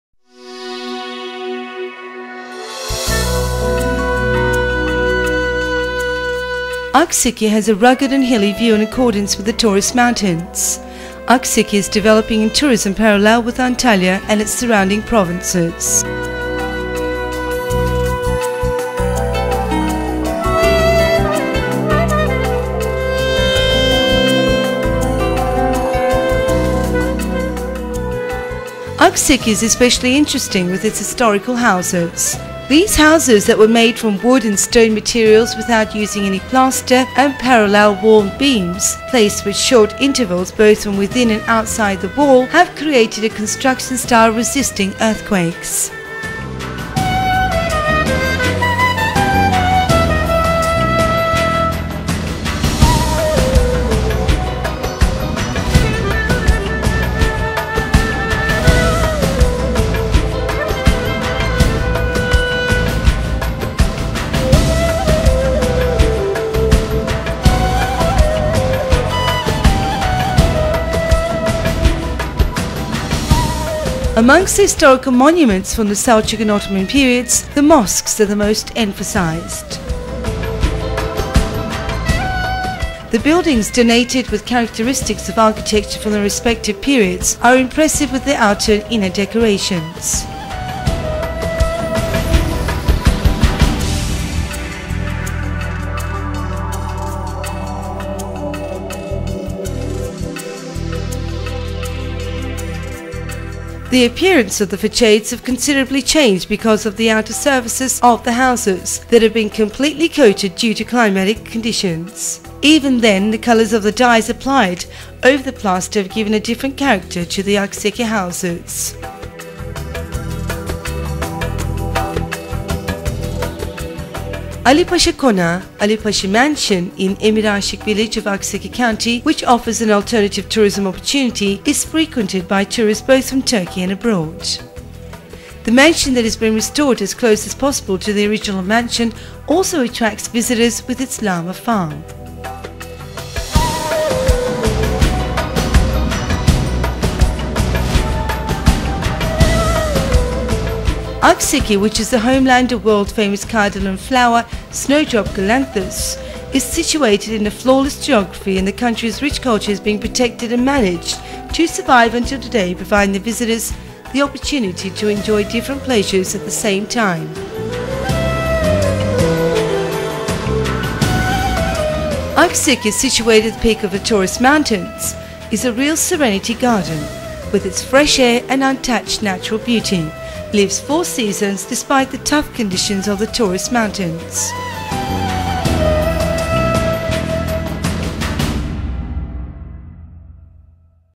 Akseki - (General Information)
Akseki

After Alanya, Akseki is the oldest district in the province of Antalya, and has an appearance that befits its location in the rugged Taurus mountains, in a forested and very rocky area. The history of Akseki extends back to the Roman era, when it was known as Marla (Marulya), and has been continually inhabited until the present day. The developments in the tourism sector in the Antalya region in recent years have been seen in Akseki as well. The area is well known for the snowdrop flower, and every years sees local and foreign visitors coming every winter to see these flowers breaking through the snow, as the first sign of spring.

In the Giden Gelmez Mountains, goats are protected and limited hunting is available around the year with the purchase of a license. Another spot frequented by visitors is the trout farming facilities in the villages of Sinan hoca and Gumusdamla. The primary game in the area is mountain goat, rabbit, bear and fox.

Other areas worth visiting are the Goktepe Highland, Giden Gelmez Mountains, Cimi Highland, Irmak Valley and the 340-metre deep Bucaklan Cave, which has only recently been discovered. Buildings of interest are the Ulu Camii and medreses.